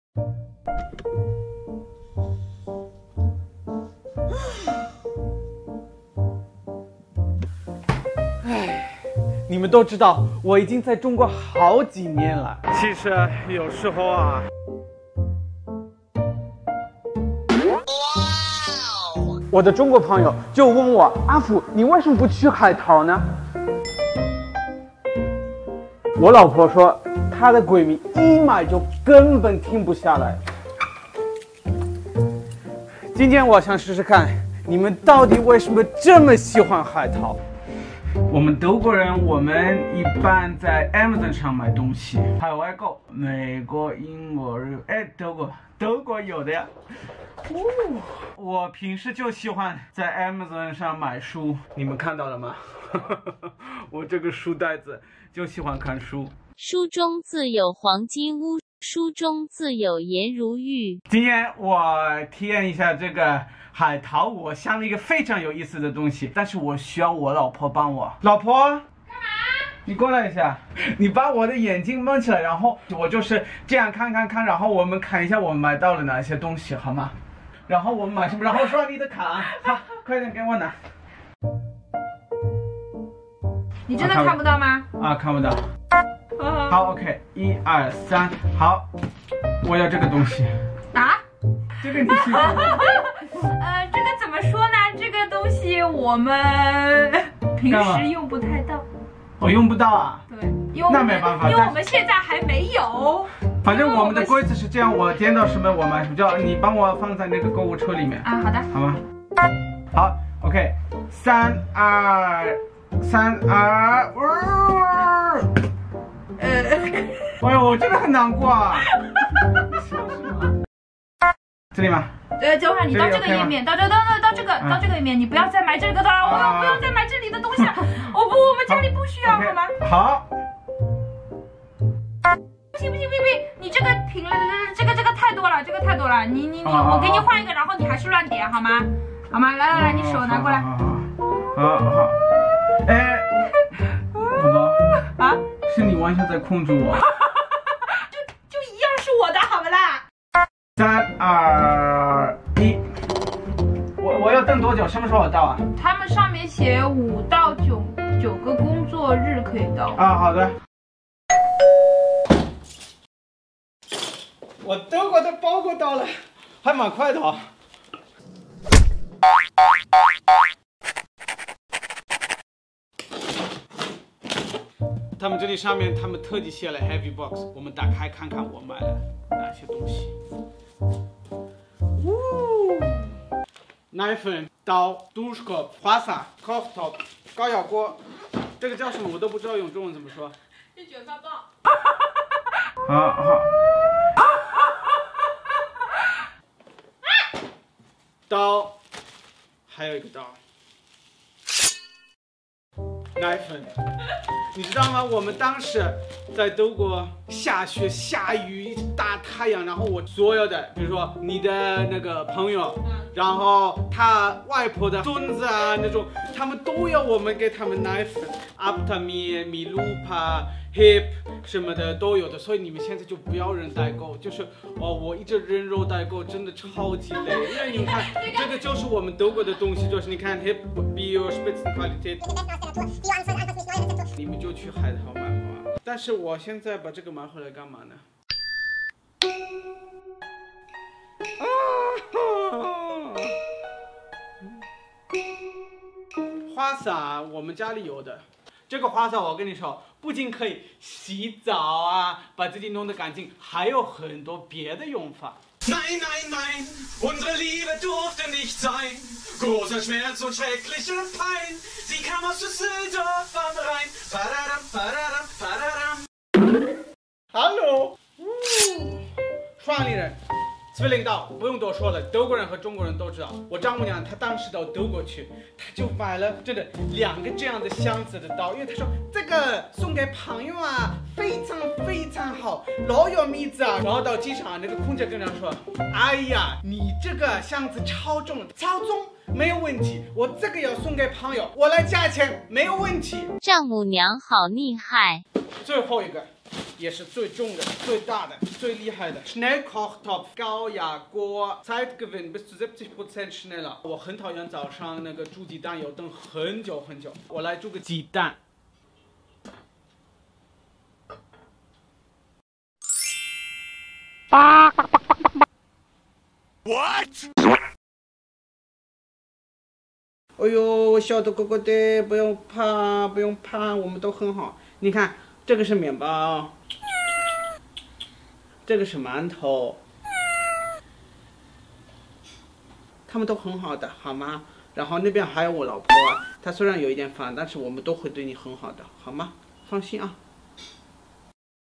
一次无厘头的海外购体验！
在中国也能这么方便的买到德国货啦！！！